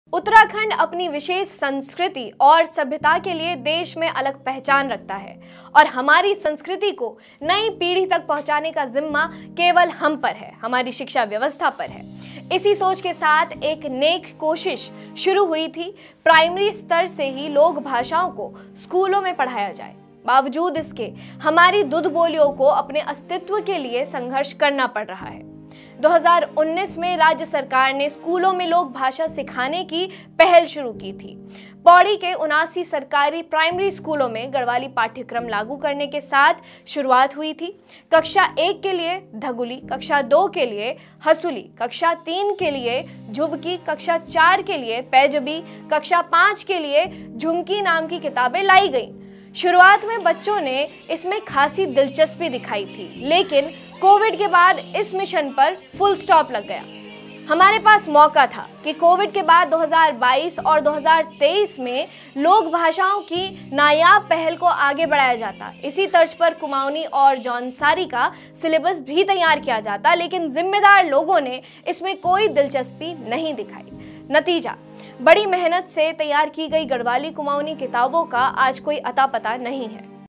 प्राइमरी स्कूलों में गढ़वाली-कुमाउंनी पाठ्यक्रम की किताबें हंसुलि, धगुलि कहां गई? Uttarakhand
प्राइमरी स्कूलों में गढ़वाली-कुमाउंनी पाठ्यक्रम की किताबें हंसुलि, धगुलि कहां गई?